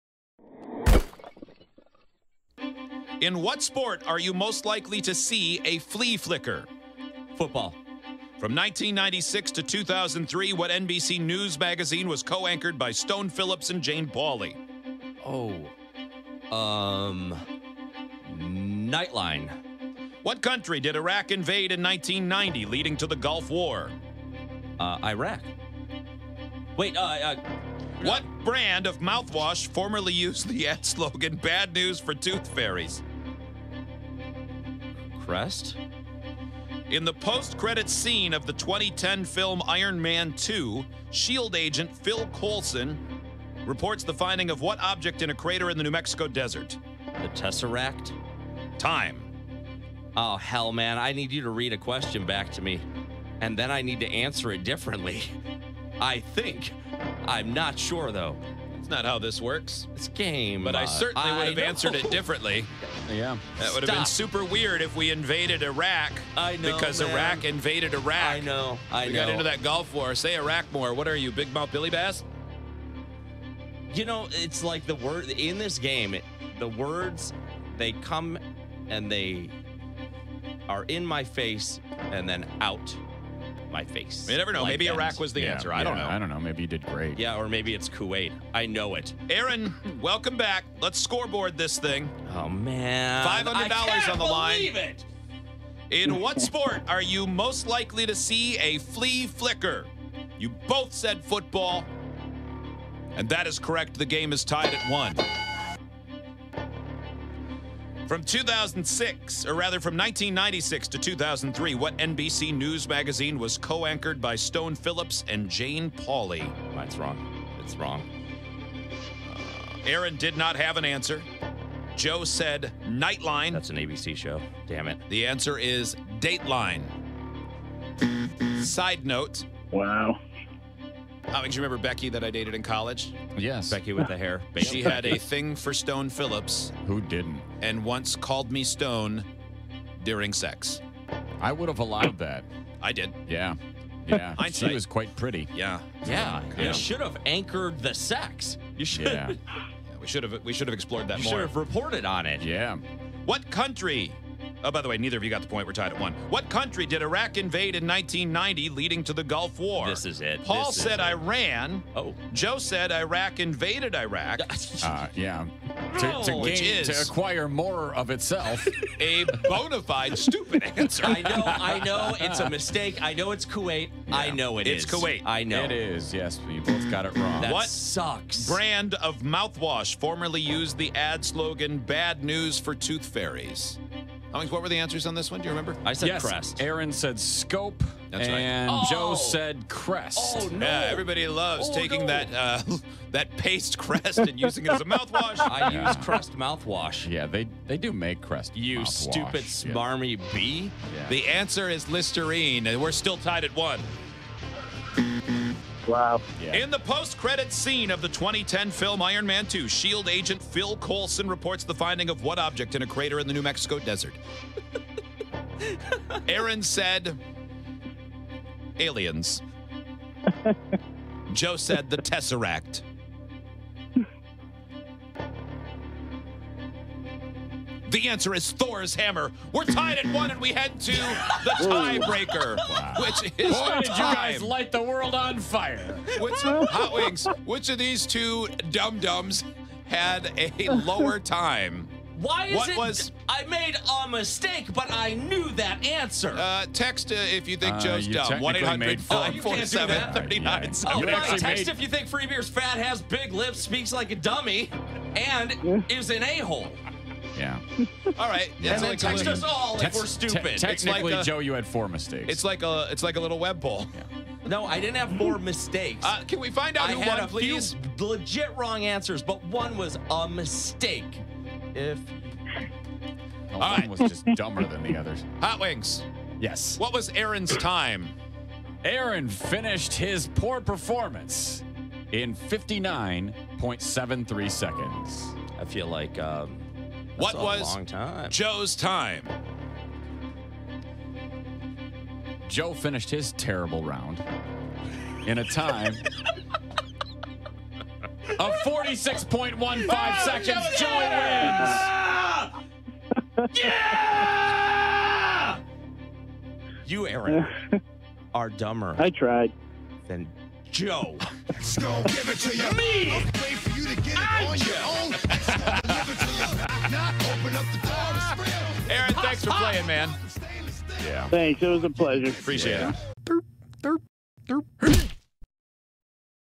Dumber Than Joe Trivia For $500
Each morning on the show, we play Dumber Than Hot Wings/Joe Trivia for a cash prize. Aaron in Portland, ME took on Joe today on a $500 round of Dumber Than The Show Trivia. Watch the clip below to find out if the pot increases, or if it resets to $100!

Get access to the webcams every day with a VIP membership